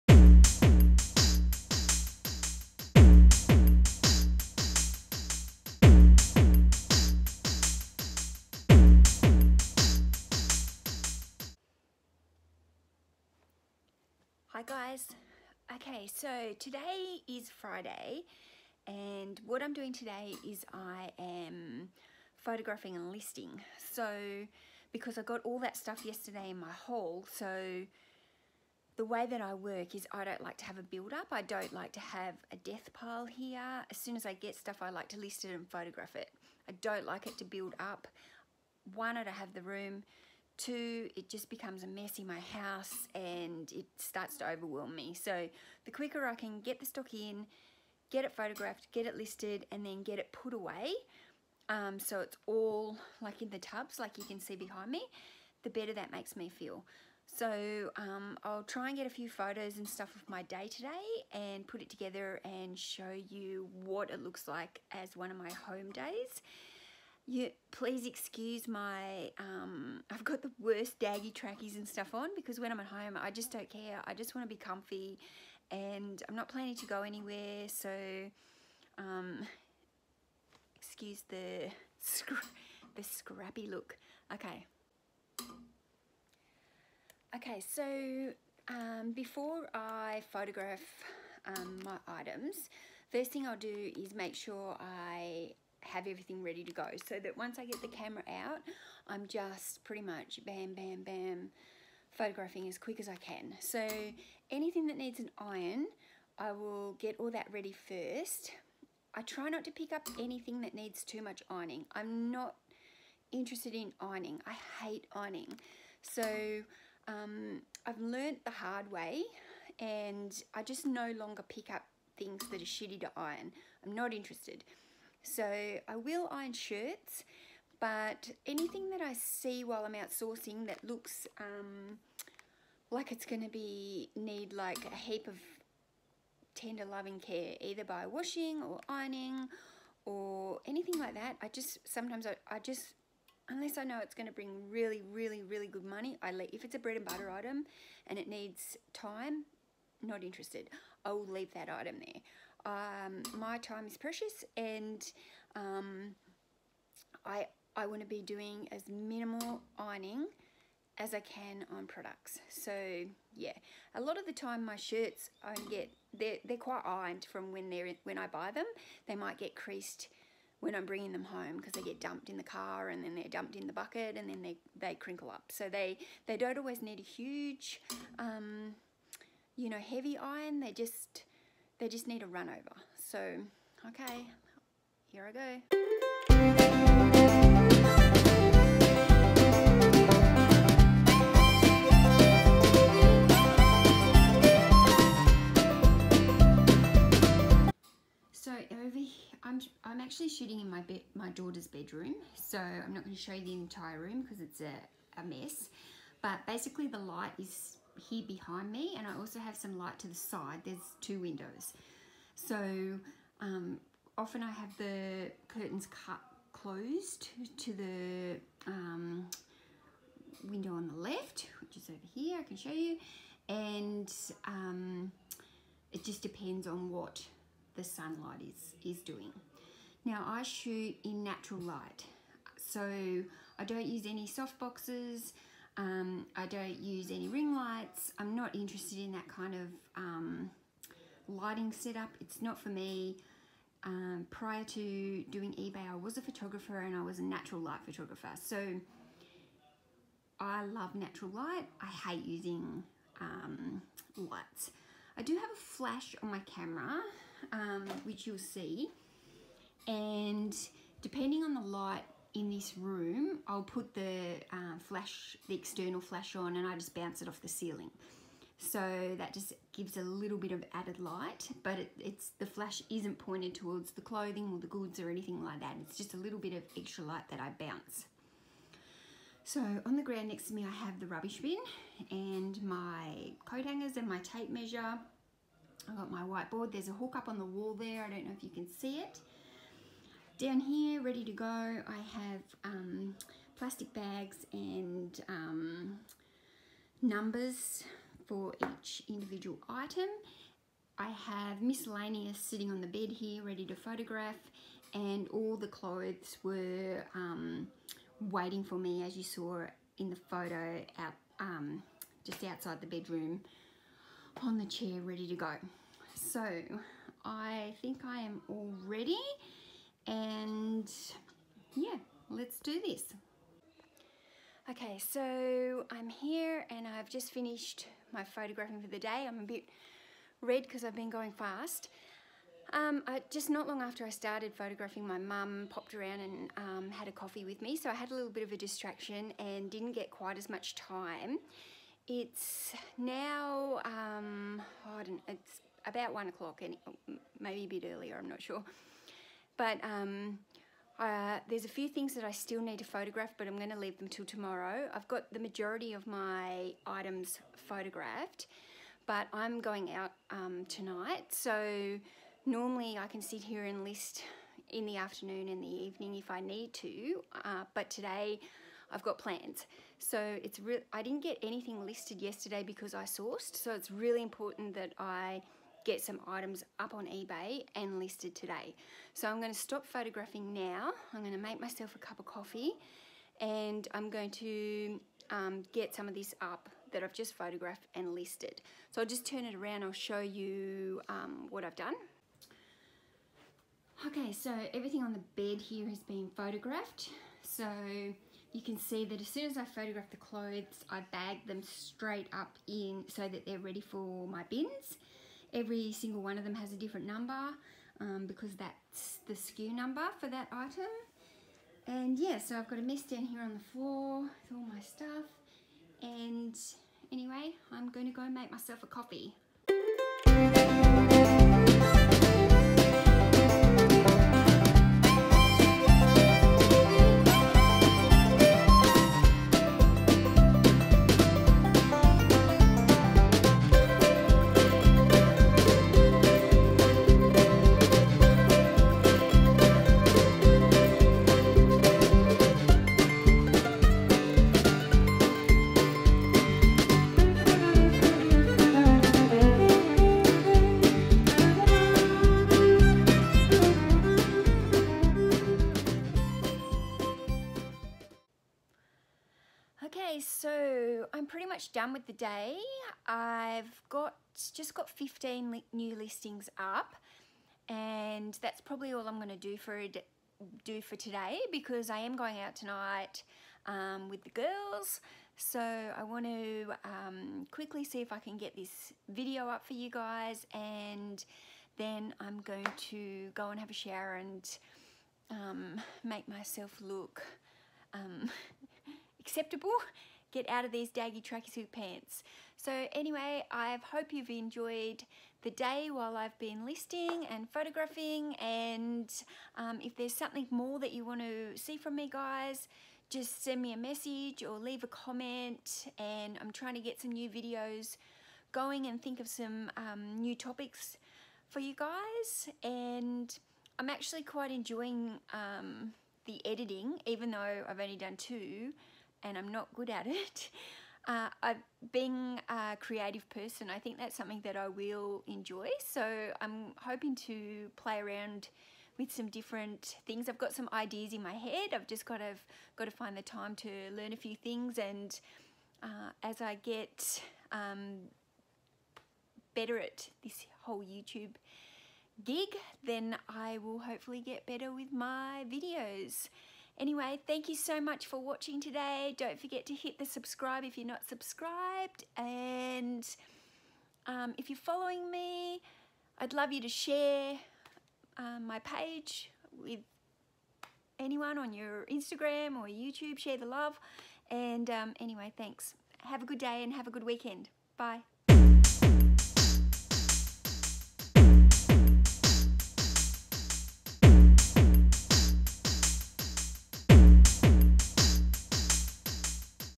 eBay Reseller | Australian Reseller Thrifts to Resell to Make Money Online
In this daily vlog I'm going to explain what I do on a day where I photograph and list my eBay items.
I am a full time reseller selling on eBay to make money while working from home. I share tips and knowledge to new resellers.

Listing Motivation on Instagram: lovetolist_

OTHER VIDEOS ON RESELLING AND EBAY